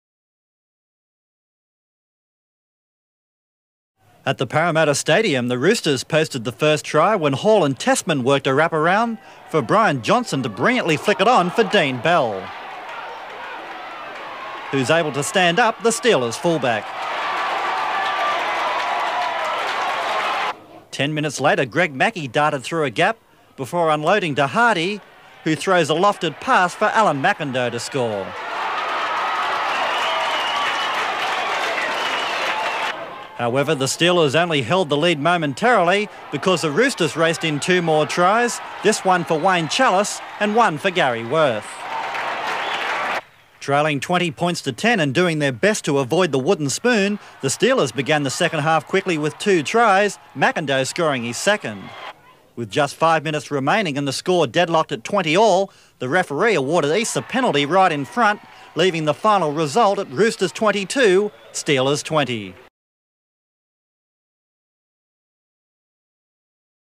Eastern Suburbs Roosters vs Illawarra Steelers | 1986, Round 25 | MATCH REPORT
Date: Sun, 24th August 1986
Referee: Phil Cooley
Venue: Parramatta Stadium
Crowd: 2,755

Eastern Suburbs 22 (D. Bell, W. Challis, G. Wurth tries; G. Wurth 4, T. Melrose goals) defeated Illawarra 20 (A. McIndoe 2, C. Macklin-Shaw tries; C. Macklin-Shaw 4 goals) at Parramatta Stadium.

Eastern Suburbs: Wurth, Challis, Prohm, Johnson, Bell; Hall, Hastings; Battese, Simpkins, Tessmann, Gillmeister, McLean, McGahan (C). Int: Melrose.

Illawarra: Macklin-Shaw, McIndoe, Mannix, Hetherington (C), Browne; Hardy, Mackey; Spring, Bolt, O'Connell, Upfield, Carberry, Worthington. Int: Greenland, McPherson, Pattison, Bradley.